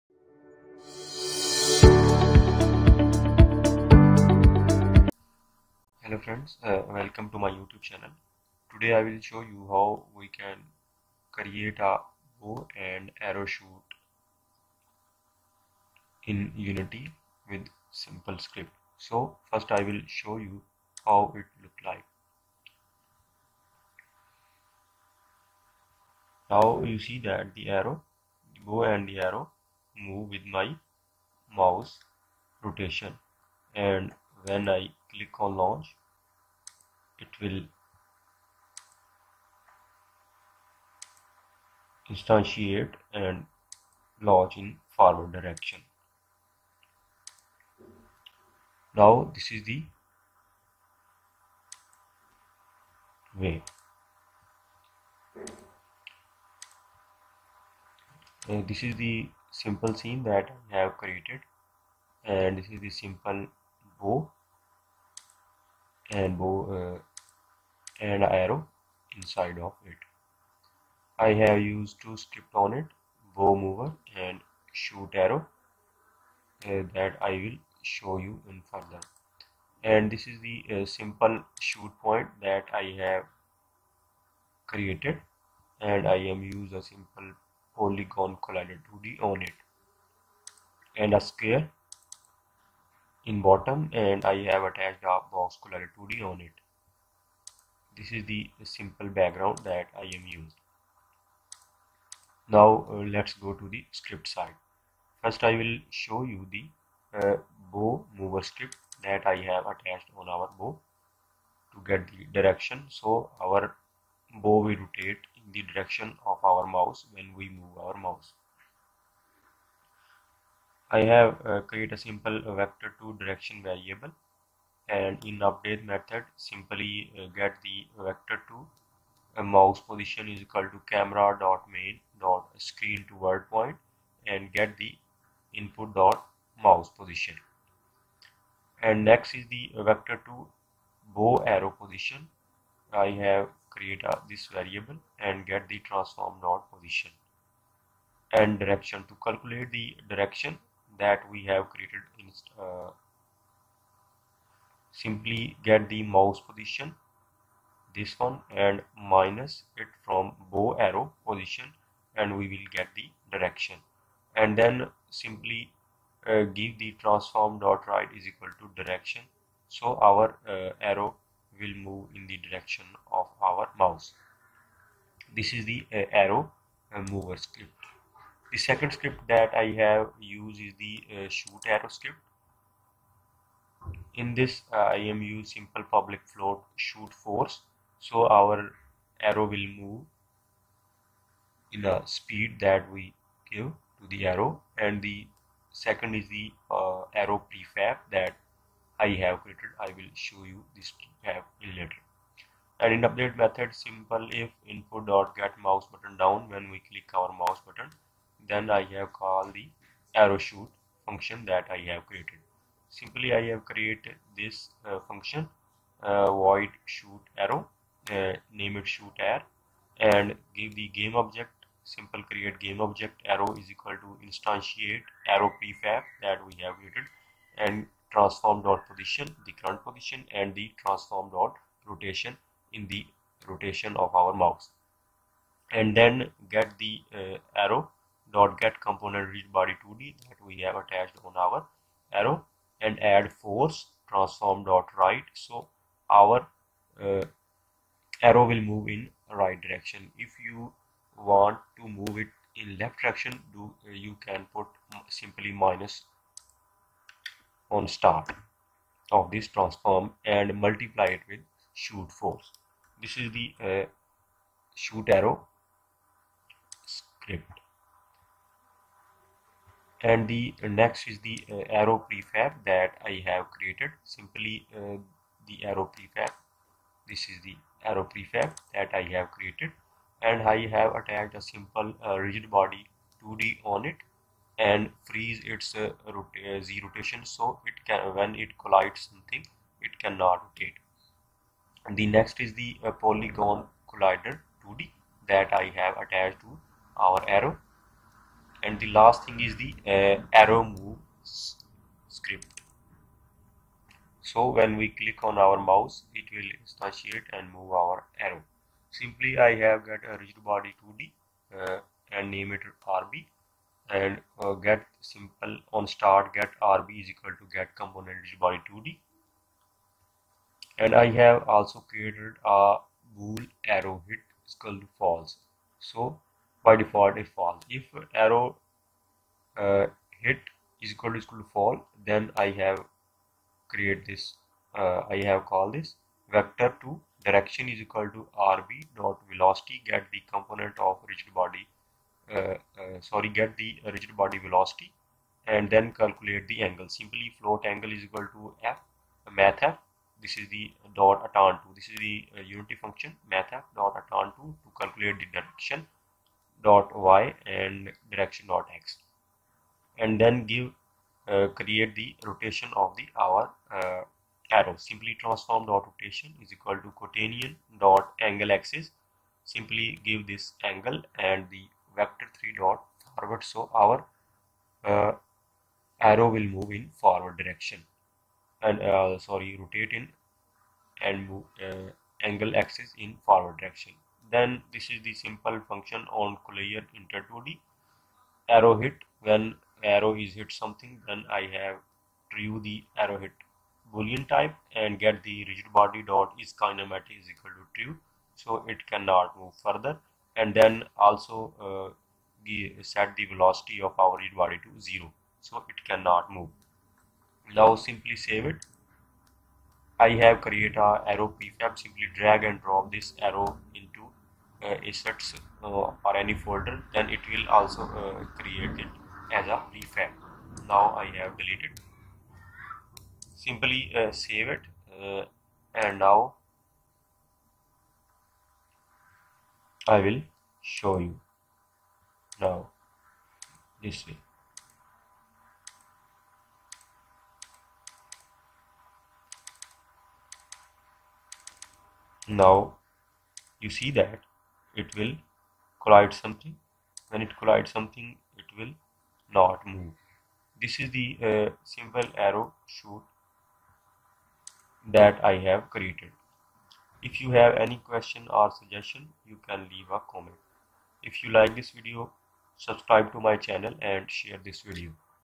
Unity Shooting Arrows |Archery Arrows that Rotate |Basic Bow and Arrow
in this video i will show you how to create Bow and Arrow Lunch in unity with mouse click and show you the scripts that are used to create it